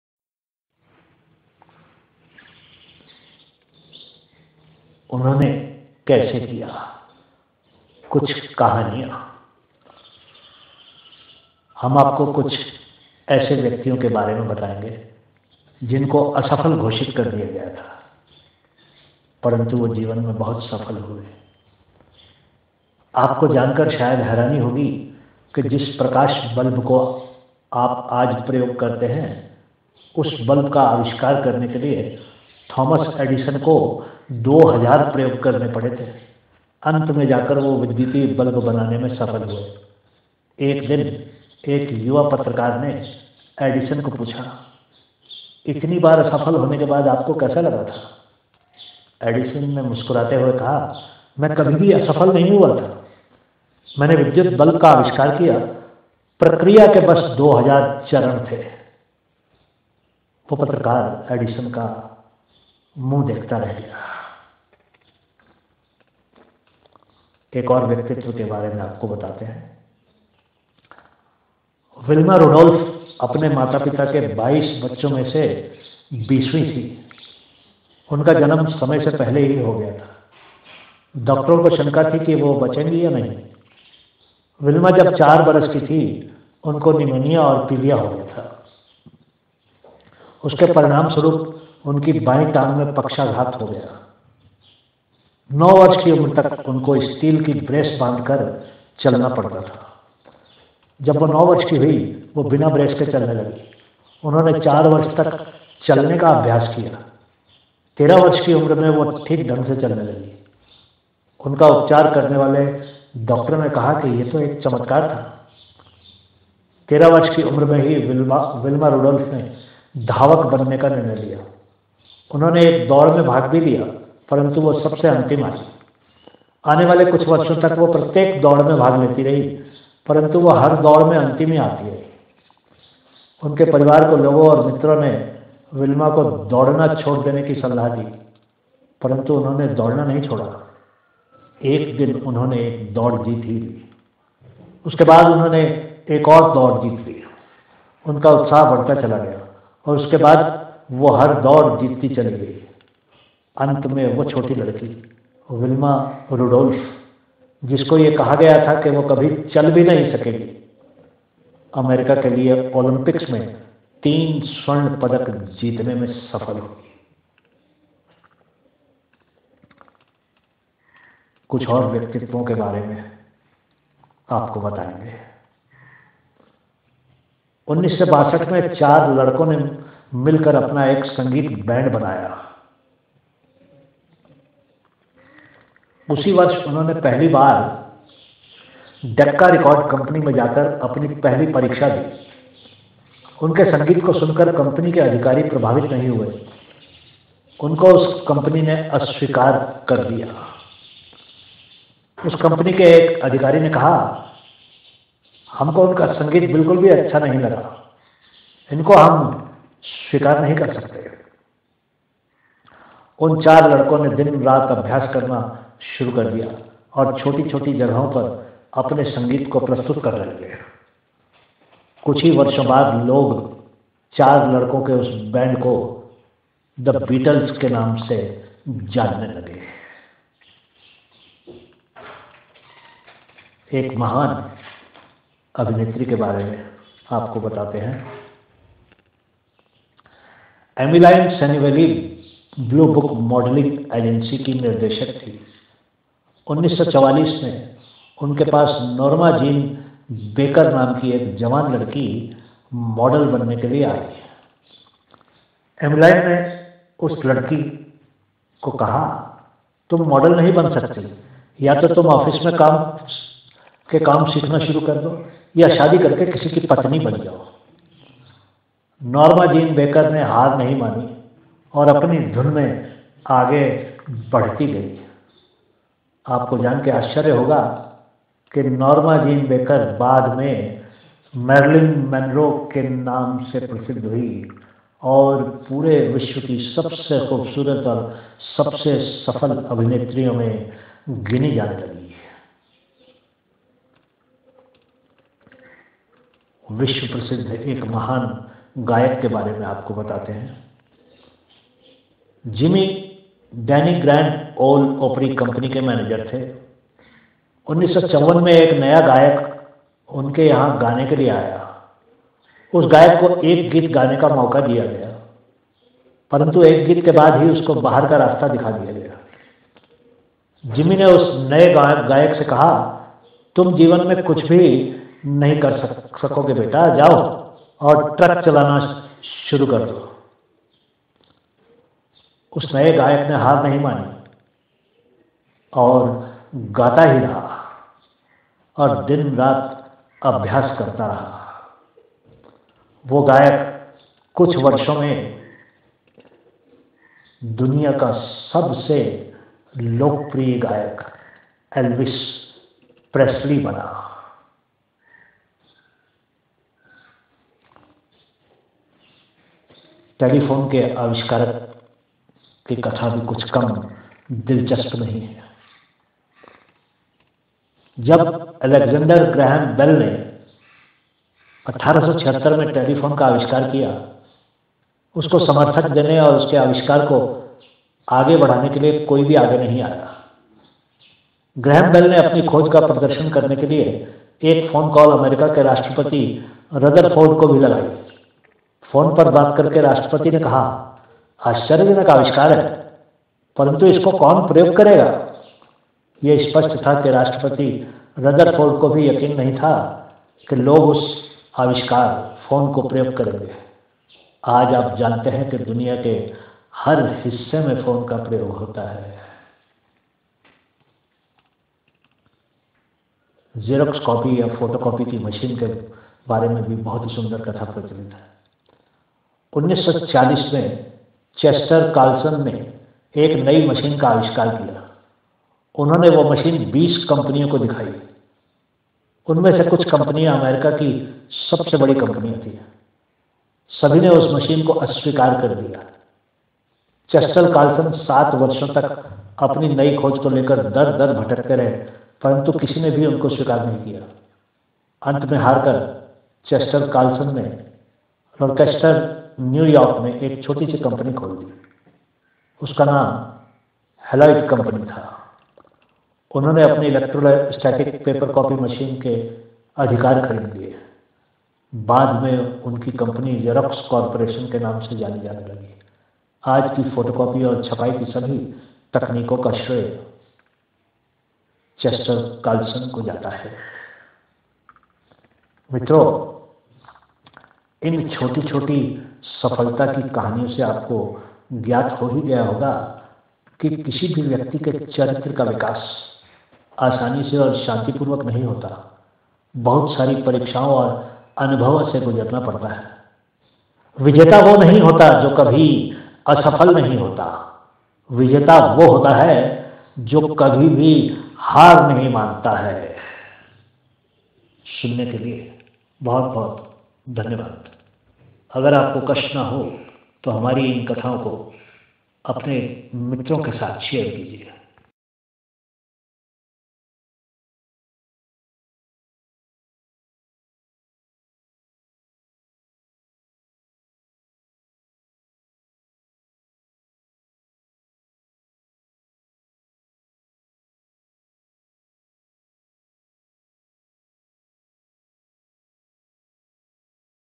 उन्होंने कैसे किया (कुछ कहानियां)
इस कहानी में जो विषय राजा शर्मा जी अपनी ही अवाज में प्रस्तुत कर रहे है वो निश्चय ही अपको सोचने पर मजबूर कर देगा. सुनकर अगर आपको ये कहानी पसन्द आए तो लाइक जरूर कर दीजियेगा. शुक्रिया.